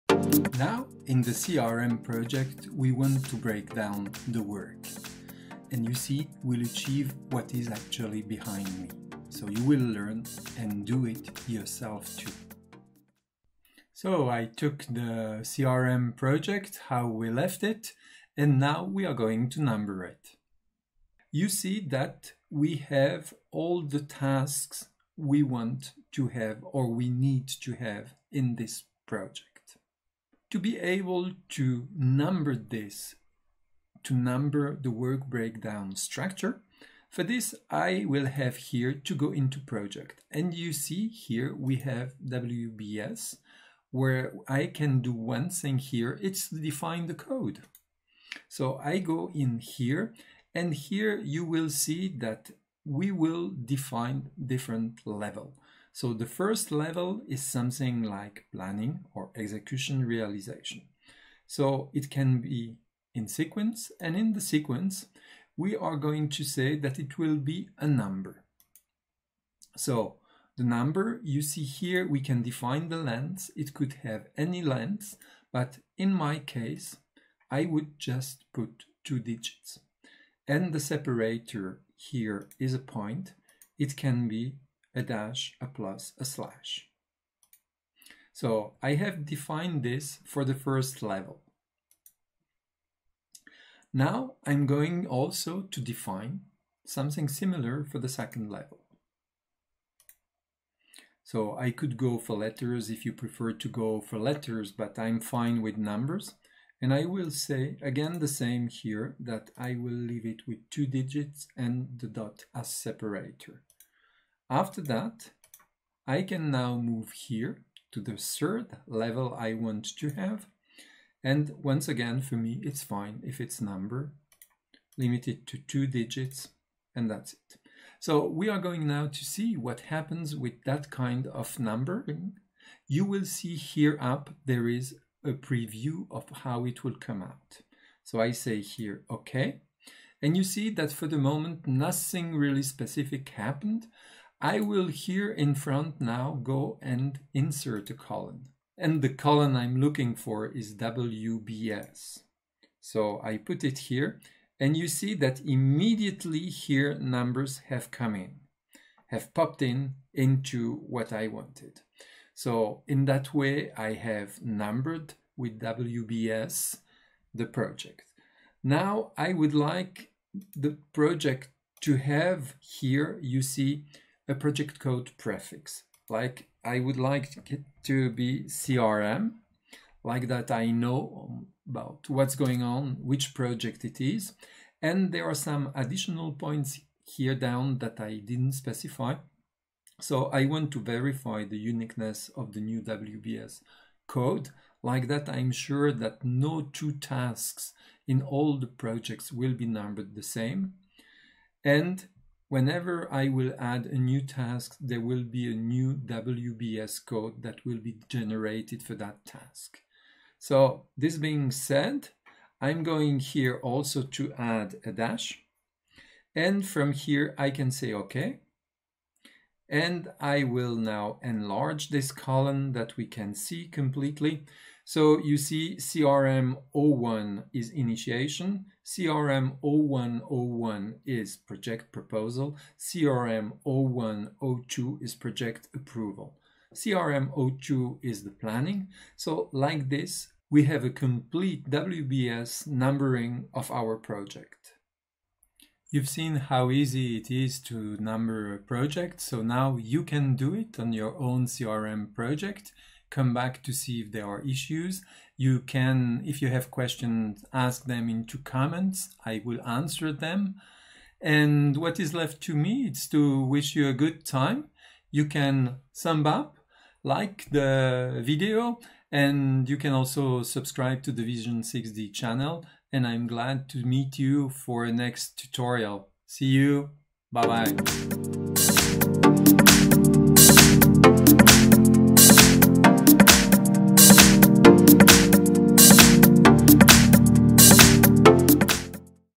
# 4.1 MS Project 2019 ● CRM Project ● How to use WBS to number tasks
We use the CRM project to show how to use WBS to number tasks in Microsoft Project 2019.
This is part of the CRM Project. It is a serie of exercises where you can do and follow the evolution of a complete project.